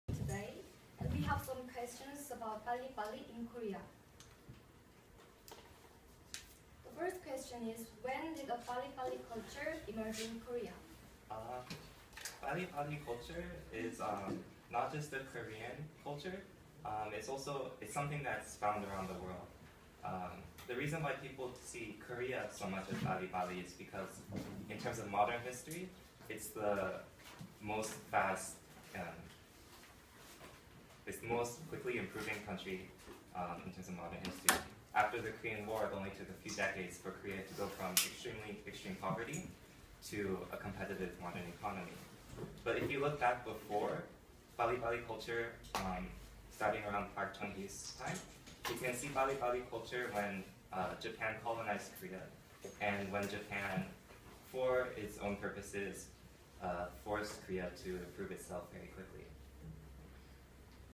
NSLI-Y Korea Cultural Explorers Project Final Presentation: Learn about Korean’s Hurrying Culture
‘Learn about Korean’s hurrying culture through the history of delivery’
NSLI-Y Korea students gathered for the last time for the final presentation on February 25th 2016.